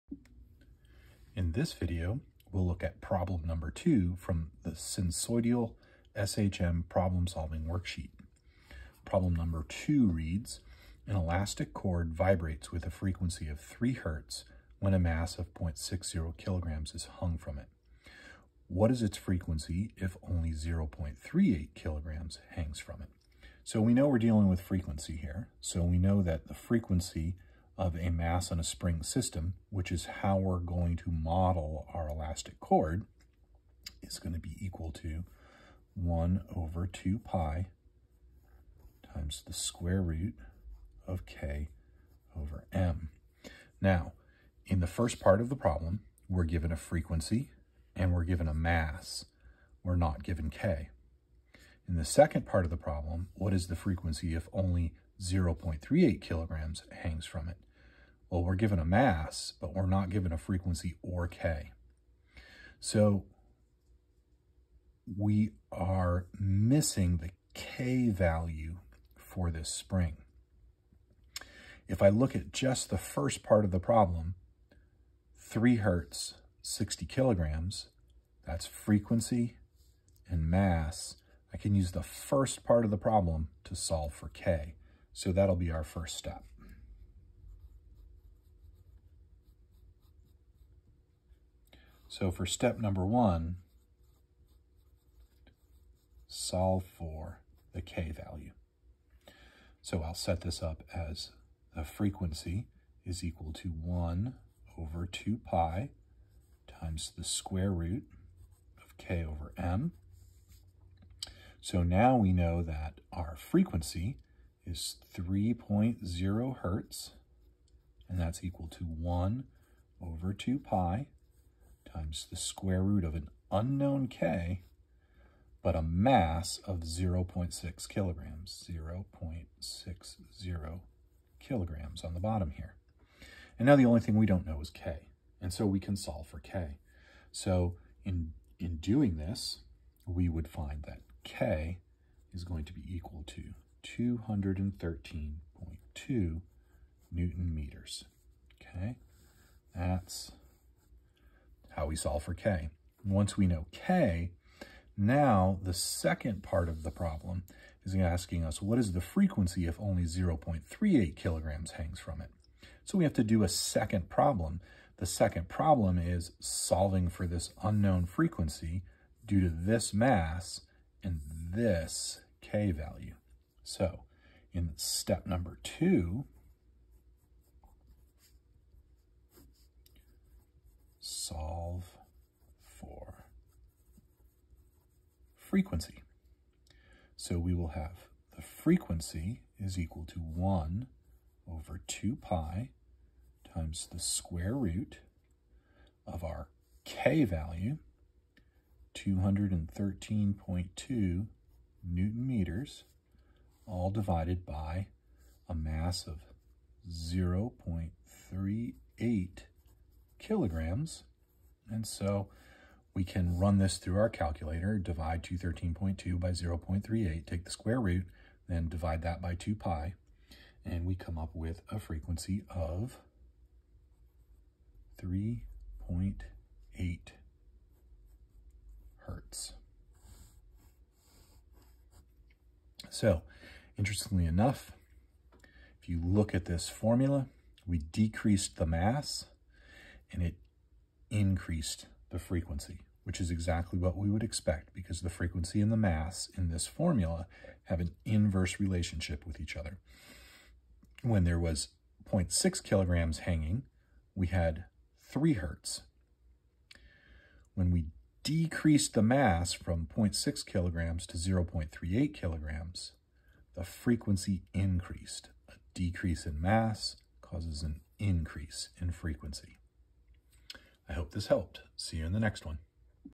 Mr. Crombie’s AP Physics 1: Sinusoidal SHM Problem Solving Worksheet: Problem 2
An elastic cord vibrates with a frequency of 3.0 Hz when a mass of 0.06 kg is hung from it. What is it frequency if only 0.38 kg hangs from it?